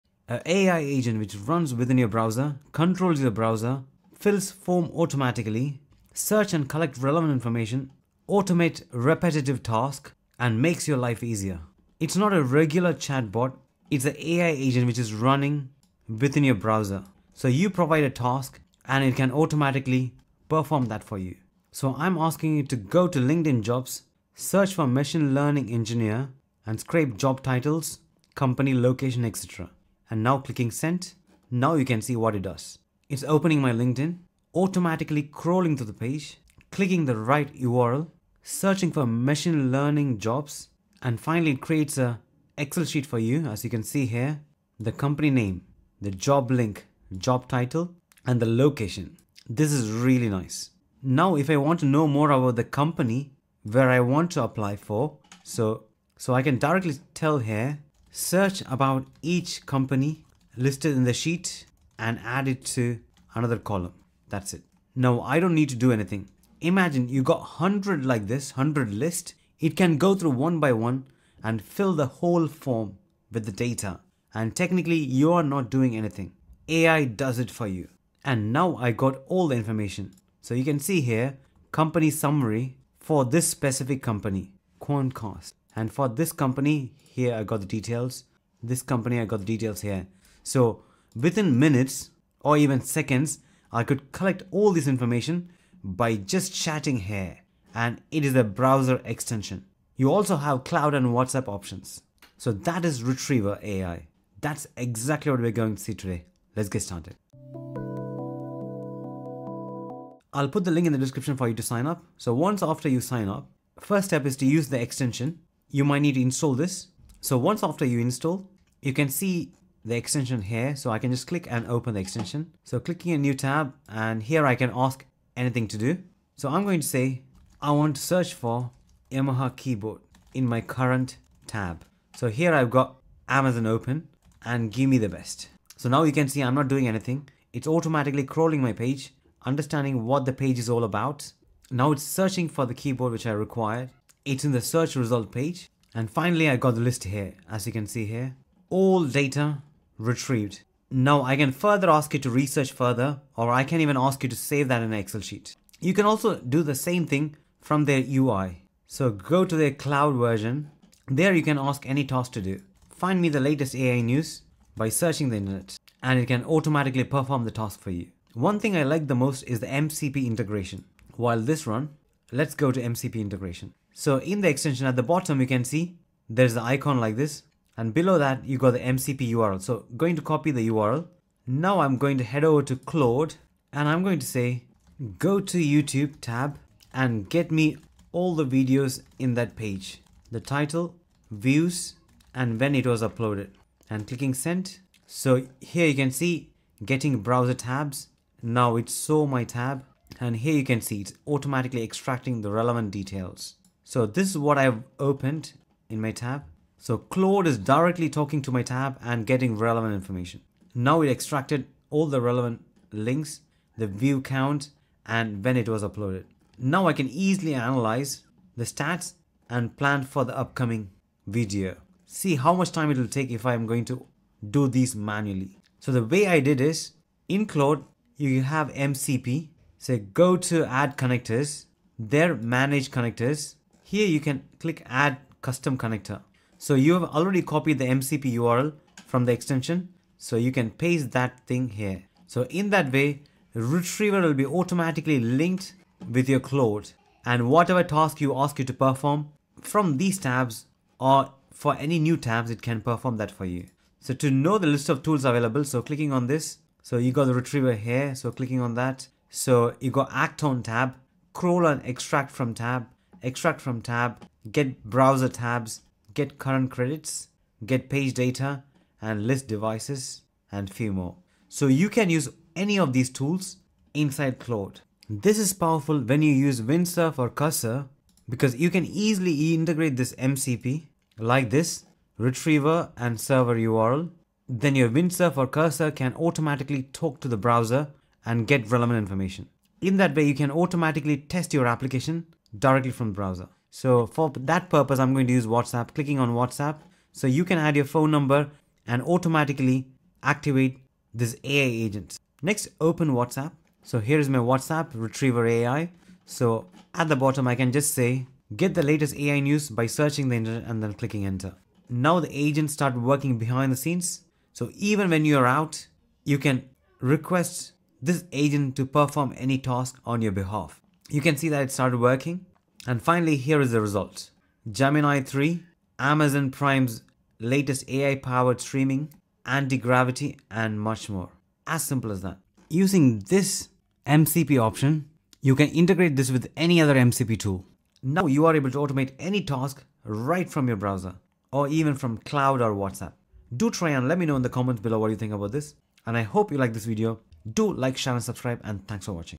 Rtrvr AI: LinkedIn Job Hunting Just Got 10x Easier With AI!
Discover Retriever AI - the ultimate browser-based AI agent that automates your online tasks! This isn't just another chatbot - it's an intelligent agent that can control your browser, fill forms automatically, scrape data, and handle repetitive tasks while you focus on what matters.

In this video, I demonstrate how Retriever AI can search LinkedIn for machine learning engineer positions, automatically scrape job details including titles, company names, and locations, then compile everything into a neat Excel sheet. But that's just the beginning - you can even ask it to research each company and add summaries to your spreadsheet, all without lifting a finger!
Key Features Covered:

Browser extension functionality for automated web tasks
LinkedIn job scraping and data collection
Amazon product search and comparison
Cloud version for internet-wide searches
MCP (Model Context Protocol) integration with Claude
WhatsApp integration for on-the-go automation
YouTube data extraction and analysis
Integration with development tools like Windsurf and Cursor

Whether you're job hunting, doing market research, or testing web applications, Retriever AI can save you hours of manual work. The MCP integration is particularly powerful for developers, allowing seamless connection between Claude, your browser, and your coding environment.
Perfect for:

Job seekers automating their search
Researchers collecting data
Developers testing applications
Anyone tired of repetitive online tasks

Here are the key timestamps for your video:

0:00 - Introduction to Retriever AI
0:21 - LinkedIn Job Scraping Demo
0:50 - Automatic Company Research Feature
1:43 - Getting Started & Installation
2:06 - Amazon Product Search Demo
2:37 - Cloud Version Overview
2:48 - MCP Integration Introduction
3:04 - Claude Integration Demo
3:47 - Setting Up MCP Connector
4:14 - Available Tools Overview
4:32 - Windsurf & Cursor Integration
4:55 - WhatsApp Integration Setup
5:07 - AI News Search via WhatsApp
5:31 - Final Thoughts & Wrap-up